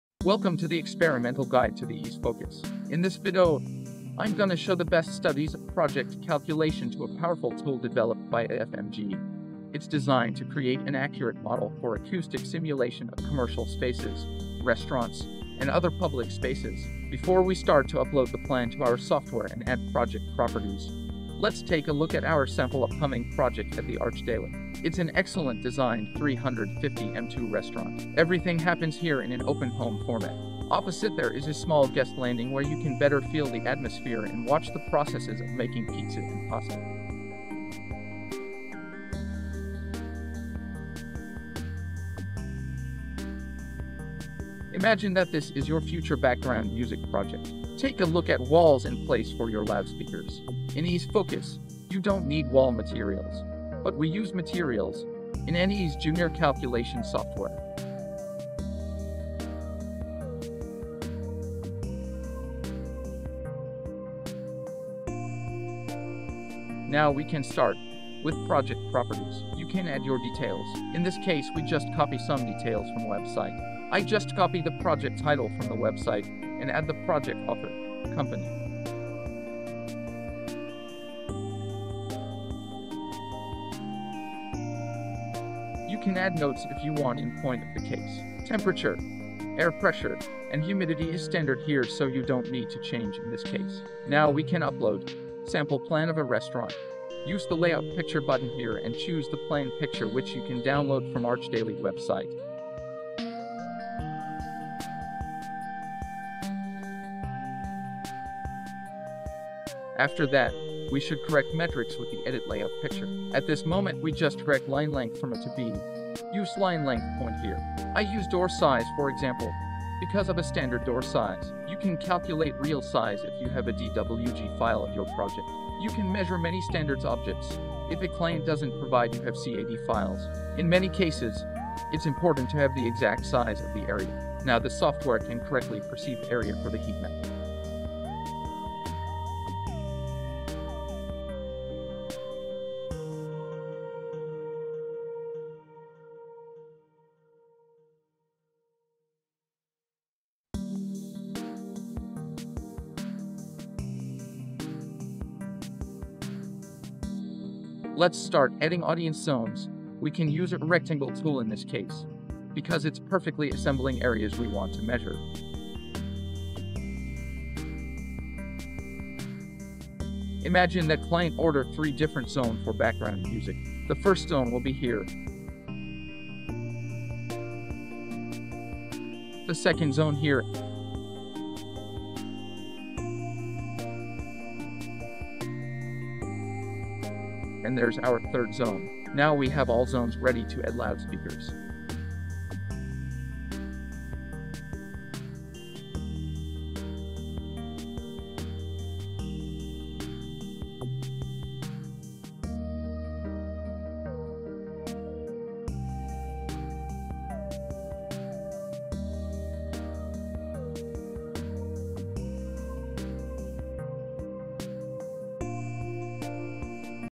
EASE Focus 3 Tutorial - Universal Sound System Modeling in 3D. Calculate Restaurant Area
Best Studies of Calculation Comercial Space Part I

EASE Focus 3 is an acoustic simulation program for 3D modeling of line arrays, sub arrays, digitally steered columns, and conventional loudspeakers. Thanks to the support from major loudspeaker manufacturers, EASE Focus 3 is free software for the end-user. Based on AFMG’s leading simulation software EASE, this concept made it possible to implement a broad scope of features and functionality.

Simulate line arrays, column speakers, point sources, and subarrays
Define audience areas, section views, and receiver locations flexibly
Calculate sound coverage and frequency response immediately
Use systems of different manufacturers within one project
Use FIRmaker optimization to compute FIR filters tailored to the venue

With EASE Focus 3, system designers can simulate the acoustical performance of the complete sound system to find the optimal setup for a given venue with multiple audience areas, curved and inclining seating. Focus 3 assists the user with many intelligent features such as Auto Splay functions, Virtual EQ, and detailed analysis of sound coverage, SPL, and frequency response.